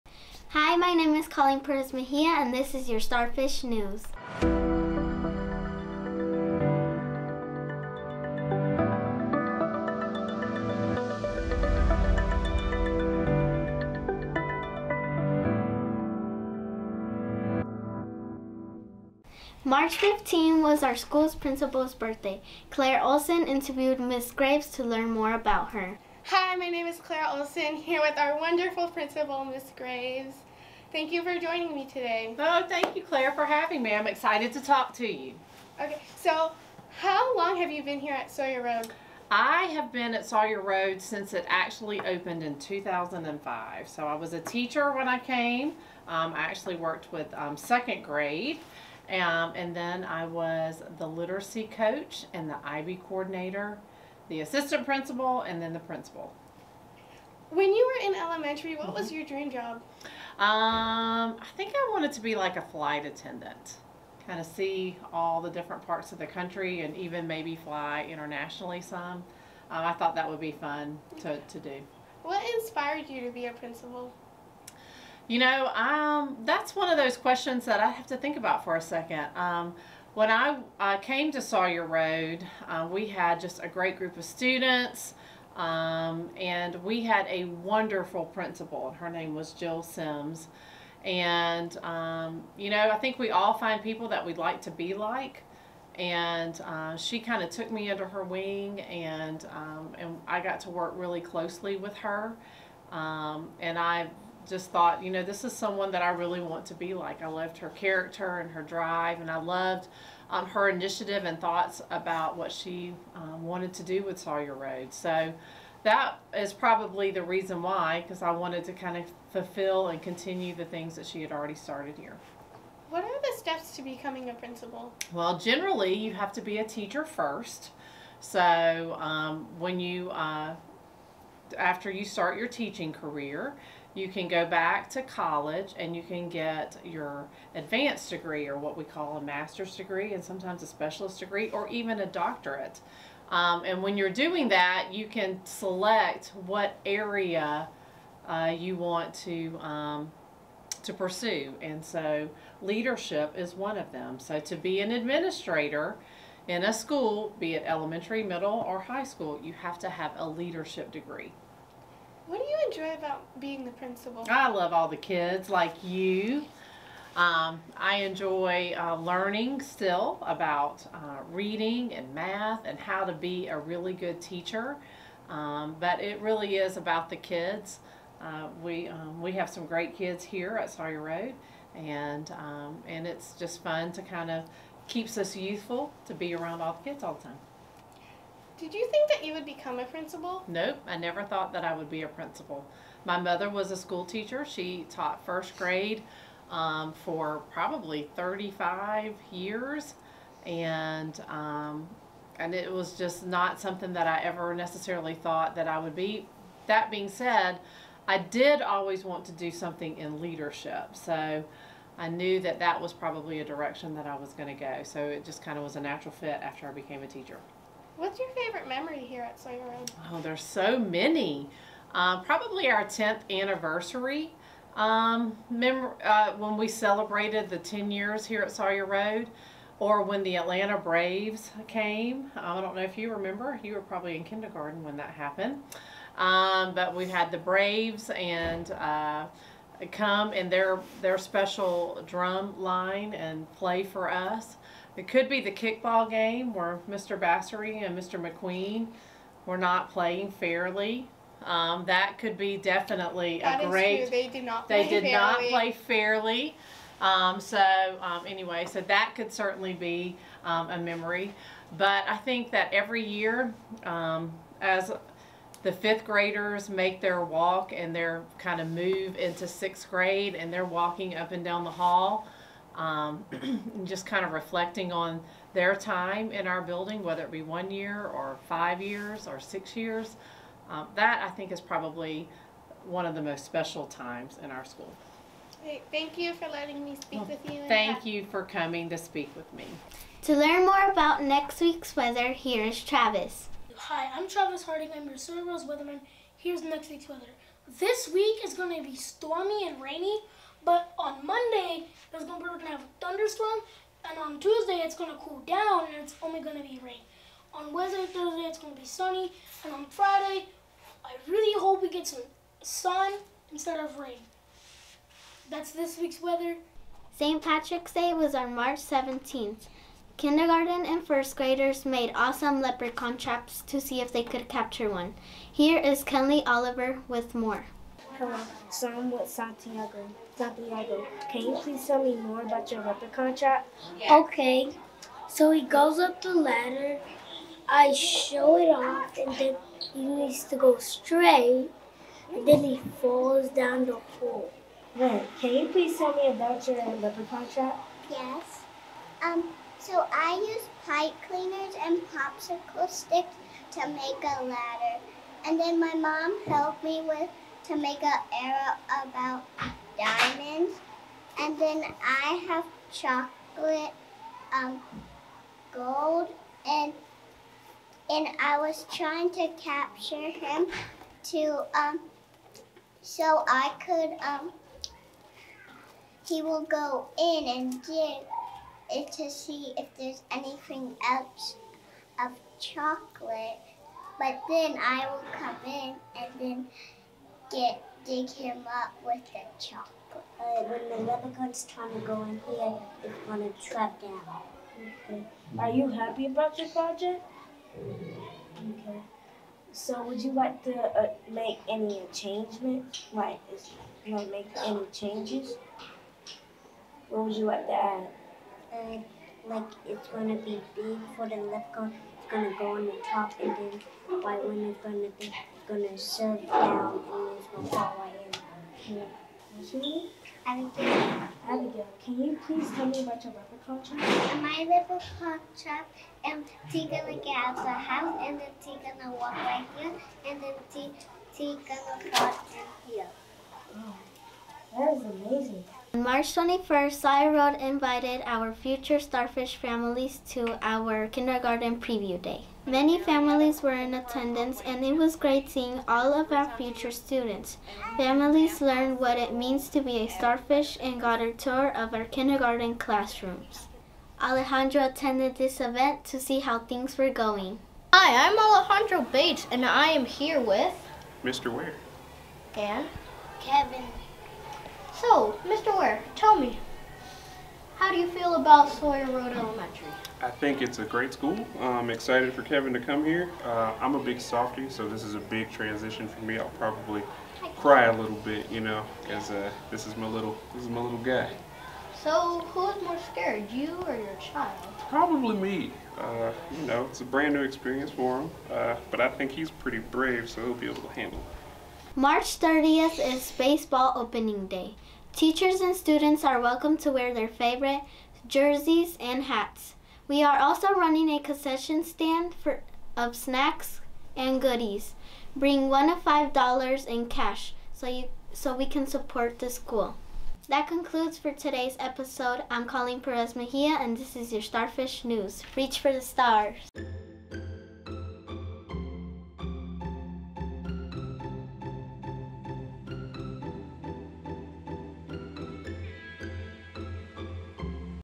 SRE News Episode 03/25/2023
On this week's episode;

Claire interviewed our school's principal, Mrs. Graves.
Kenley checked out our Kindergarten Leprechaun traps.
Alejandro attended our Kindergarten Preview Day.
Travis gave our next week's weather forecast.